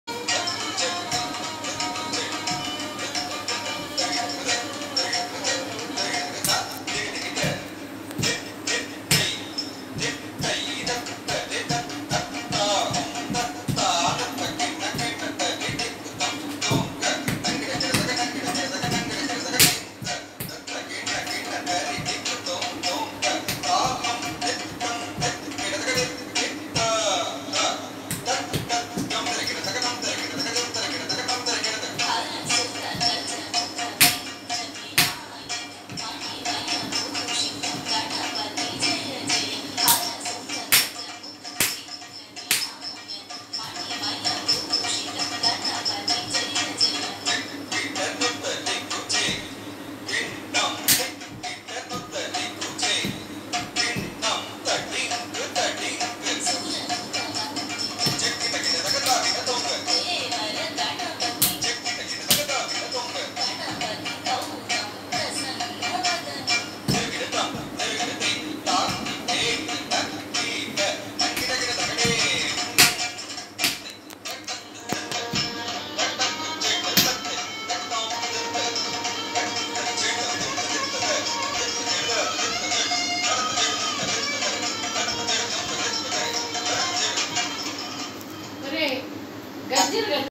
ICMDA Online Competitions 2020 Participants videos ID- 242
NAME: M. Sukruthi
CITY: Nellore
COUNTRY: India
ART FORM: KUCHIPUDI